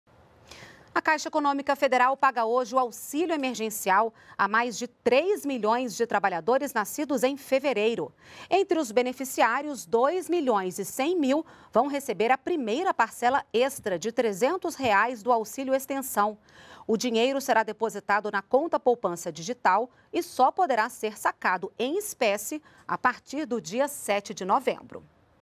Nascidos em fevereiro recebem hoje o Auxílio Emergencial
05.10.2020 - A Caixa Econômica Federal paga hoje o Auxílio Emergencial a mais de 3 milhões de trabalhadores nascidos em fevereiro. Entre os beneficiários, 2 milhões e 100 mil vão receber a primeira parcela extra de 300 reais do Auxílio Extensão.